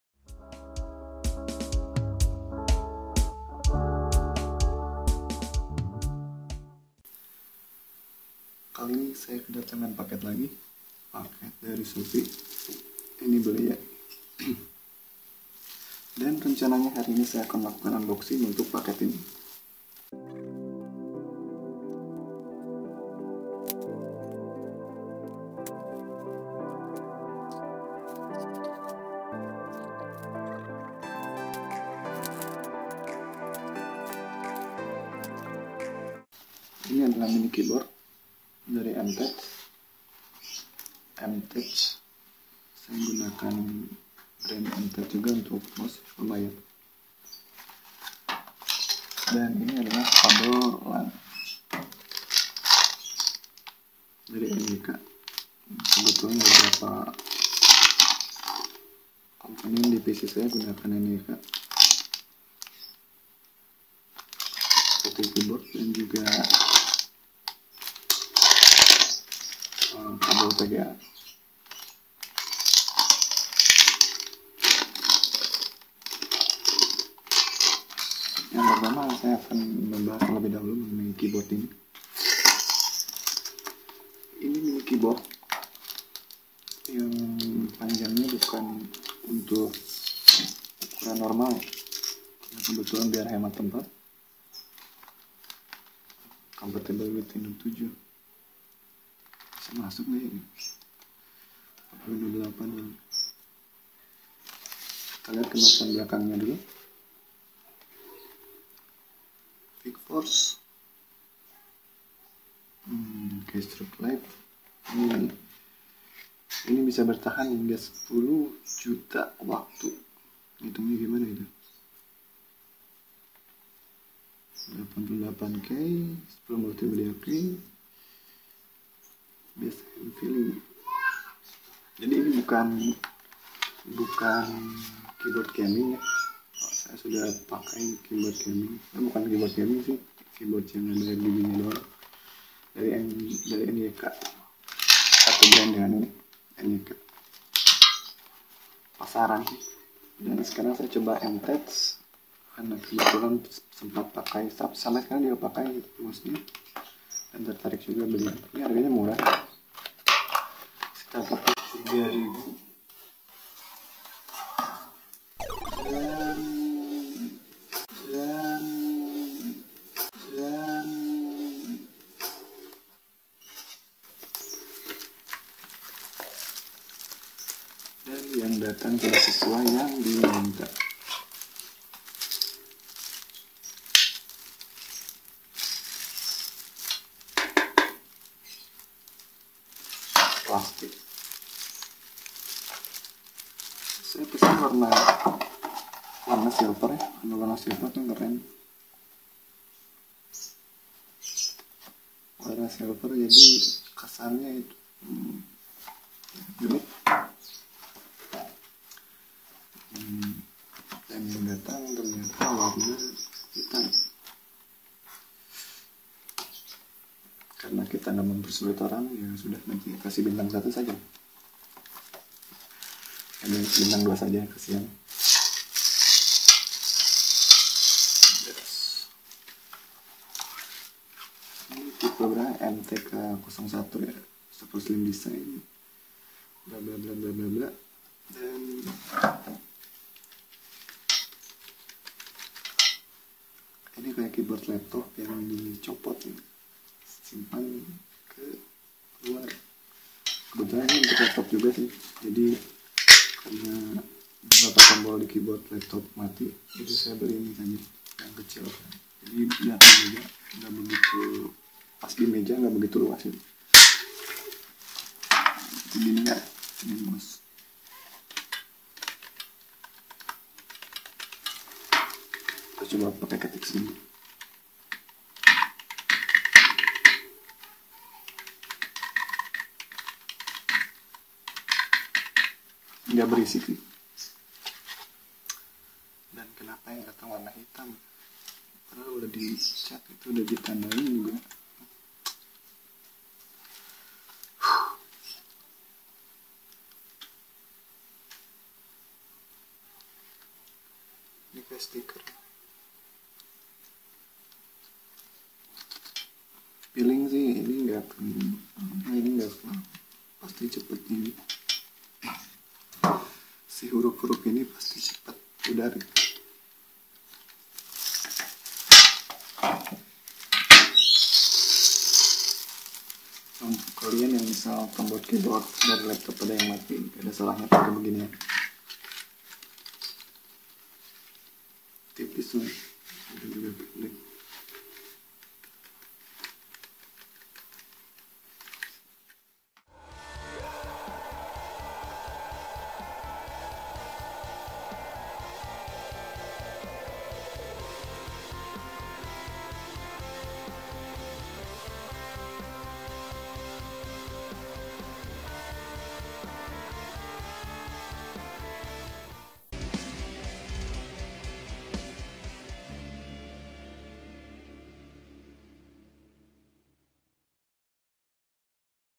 Unboxing Mini Keyboard For Laptop, Review Mini Keyboard M-Tech MTK 01
Sebelumnya saya meminta maaf jika kualitas gambar dan suara pada video ini sangat buruk.

Intinya saya akan menunjukan pada kalian mengenai Mini Keyboard M Tech MTK 01.

Bentuknya unik karena seperti keyboard laptop yang di copot. Selain itu, harganya pun murah yakni hanya 45 ribuan.

Tetapi sayangnya keyboard yang kami dapat tidak sesuai dengan apa yang kami pesan terutama pada bagian warna. Padahal kami sudah melakukan chat toko dan penjual menyatakan ready dan siap. kami pun sudah memberikan catatan pada pesanan agar lebih jelas.

Tetapi ya sudah, tak apa jika cara jualannya seperti itu, kami tidak akan rekomendasikan tokonya dan juga tentunya tidak akan beli lagi di sana.